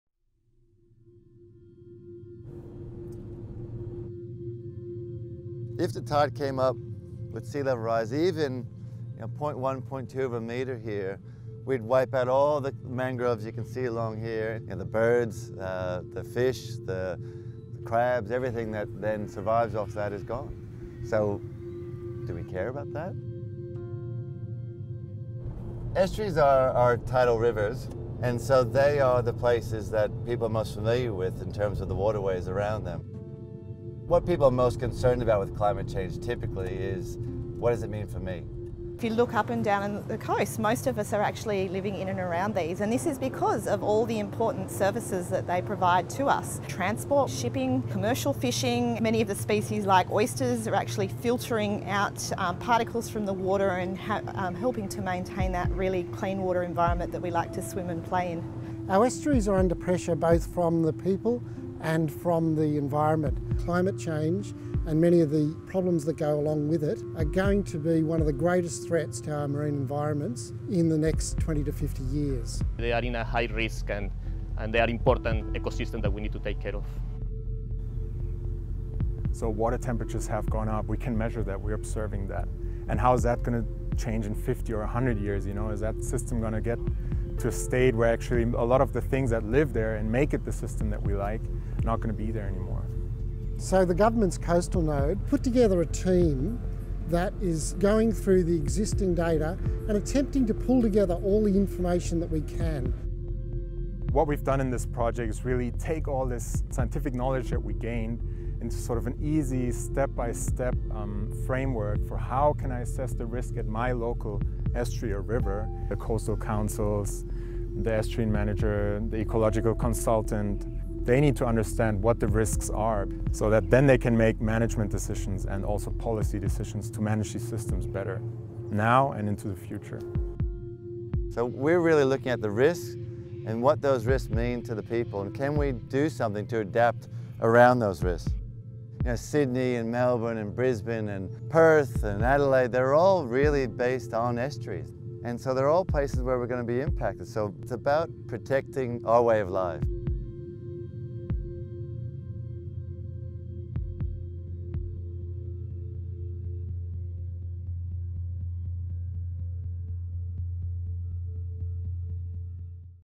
Our Estuaries
With more than two-thirds of the state’s population living in estuarine catchments, there is no doubt about the value that estuaries hold for our society. However, these systems are facing an uncertain future in a world of rapidly changing climate.